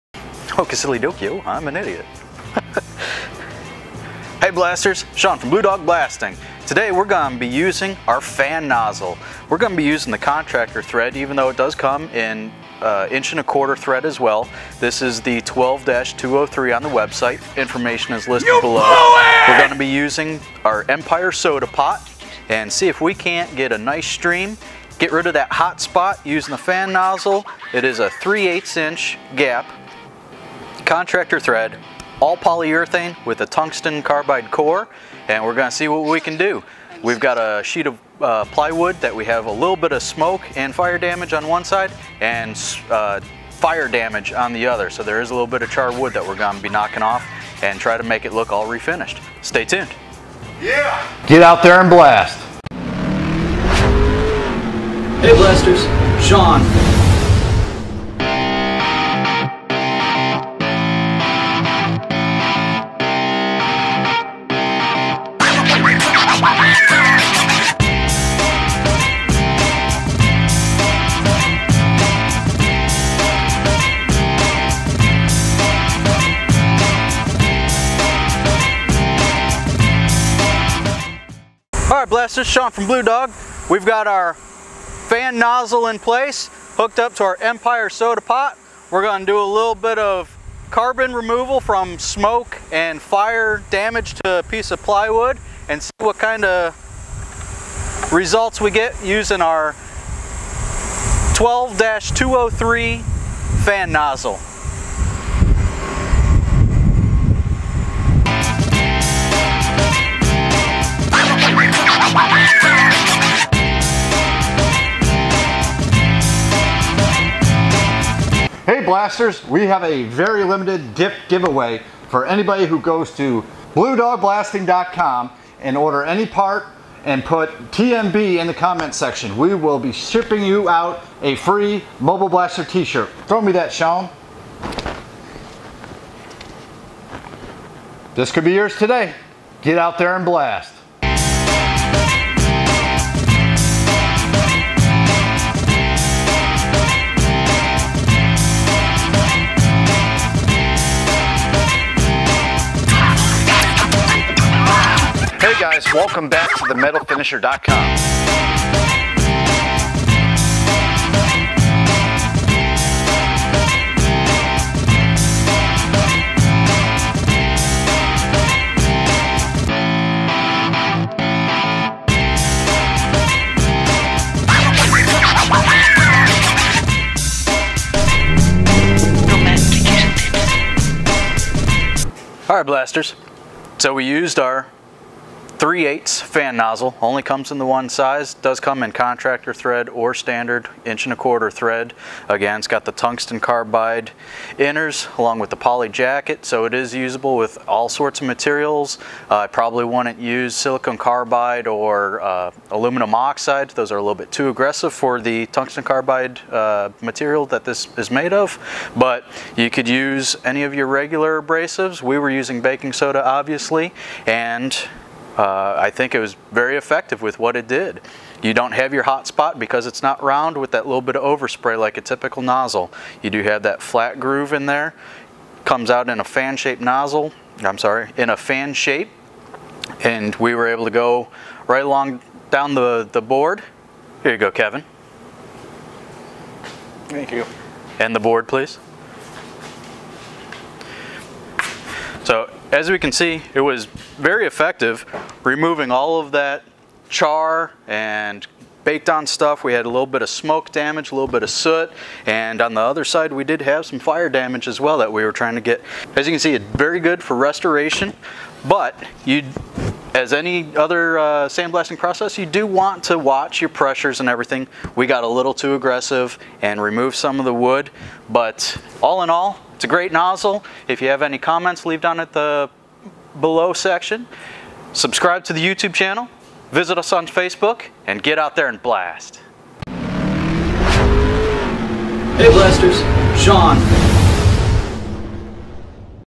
Using a Fan Nozzle and Empire Soda Pot with Armex baking soda
We used our 12-302 Tungstend Carbide Fan Nozzle with 50mm Contractor Threads to blast with Armex baking soda out of an Empire soda pot.  We were removing fire and smoke damage from wood to prove the effectiveness of these products.  Even though the fan nozzle does not have a hot spot like traditional round nozzles, we got a little too aggressive in spots and removed some wood.  Other than that, we think the experiment was very effective and we were happy with the performance of the fan nozzle.